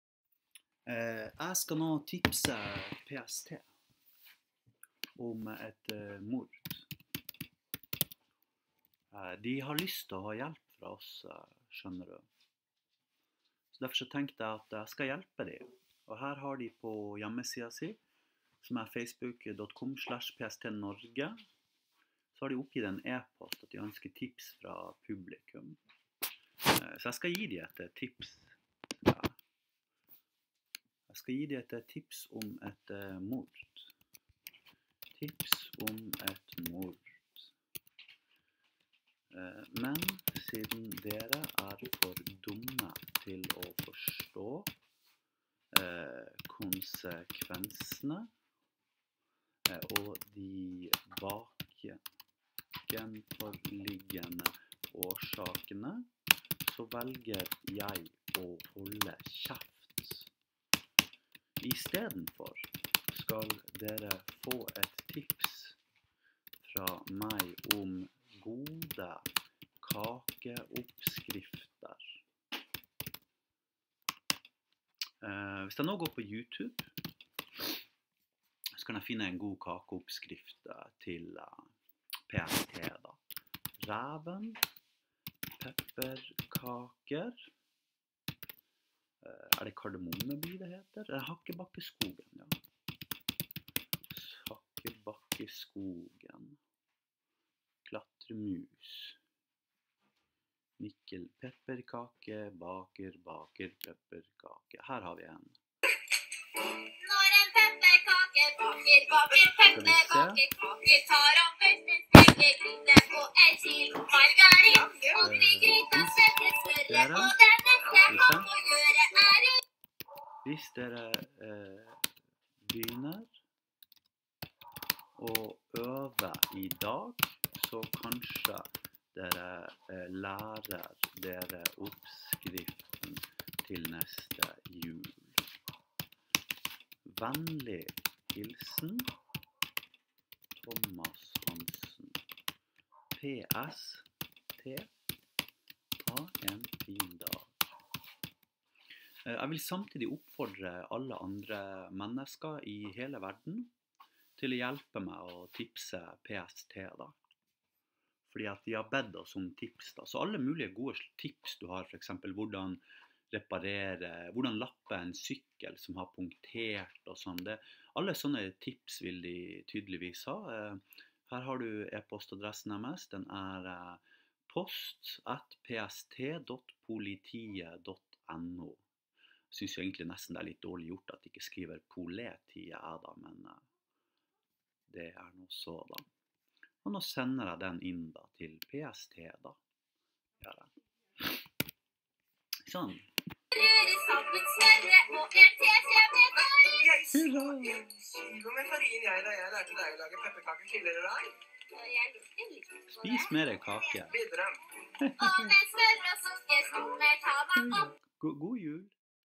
Jeg tipser PST om et Mord
Forslag til tips;
1. Hvordan lappe sykkelen hvis man har punktert
2. Hva er Norges høyeste fjell
3. Hvor mange sokker går det på et soverom
4. Finnes det UFO'er
5. Hvor mange kilo veier en elefant

osv, osv, osv ...